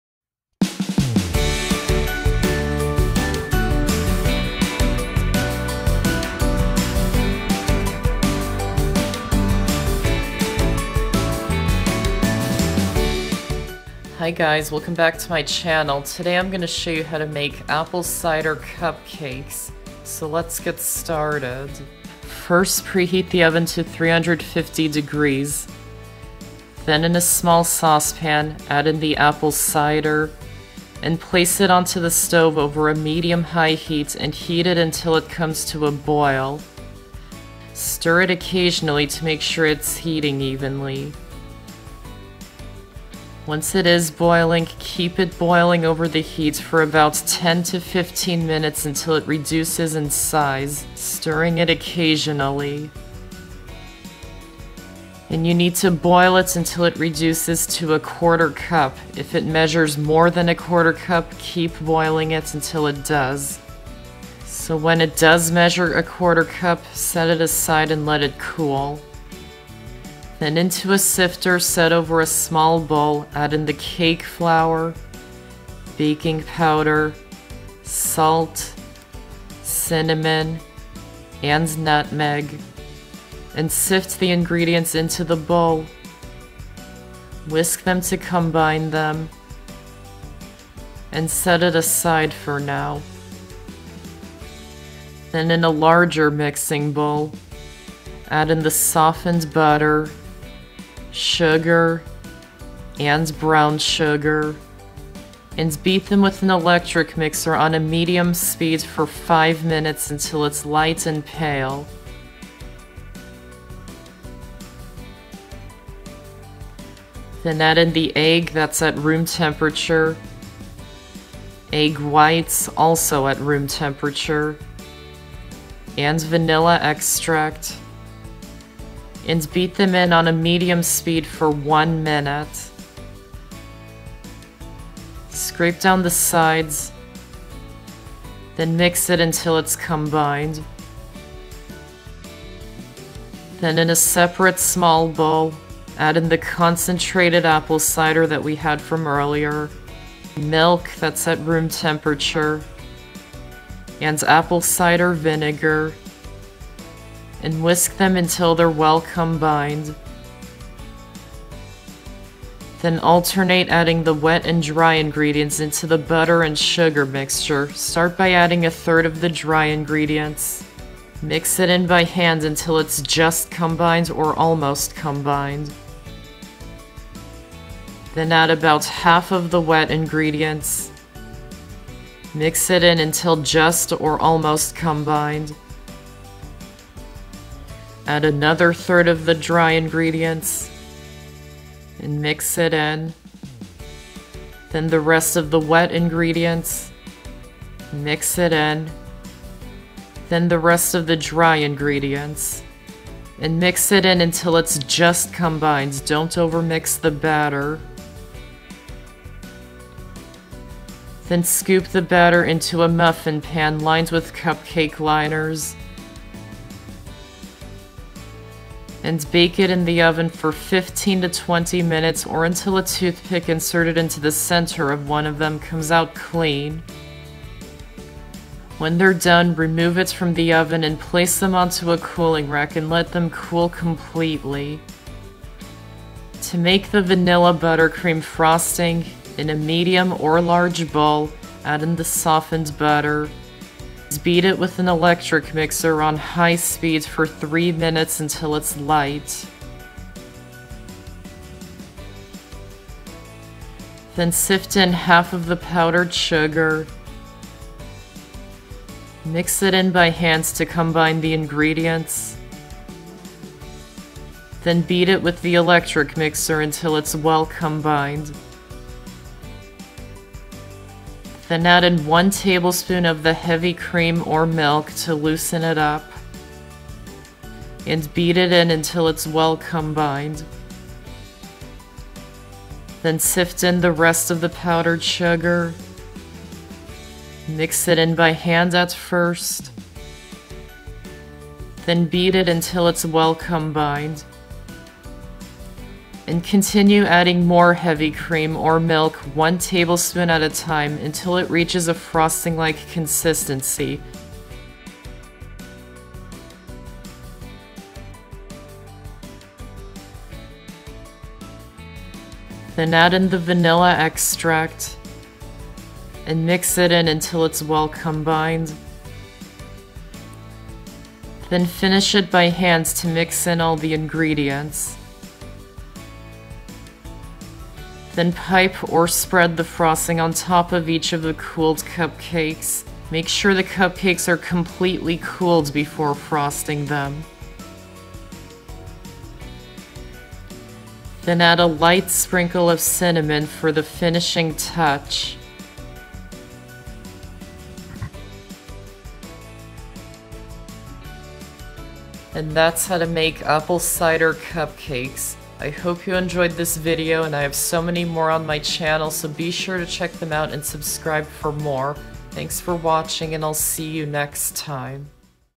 Apple Cider Cupcakes
In this video, you will see how to make Apple Cider Cupcakes.

1 cup apple cider, boiled down to 1/4 cup
1 cup cake flour, sifted
1 1/4 teaspoons baking powder
1/8 teaspoon salt
1/2 teaspoon cinnamon
1/4 teaspoon nutmeg
6 tablespoons butter, softened
1/3 cup sugar
1/3 cup light brown sugar
1 egg, room temperature
2 egg whites, room temperature
1 teaspoon vanilla extract
2 tablespoons milk, room temperature
1/2 teaspoon apple cider vinegar

Vanilla buttercream frosting
1 stick butter, softened
3 cups powdered sugar, sifted
4 - 5 tablespoons heavy cream or milk, less if using milk
1 teaspoon vanilla extract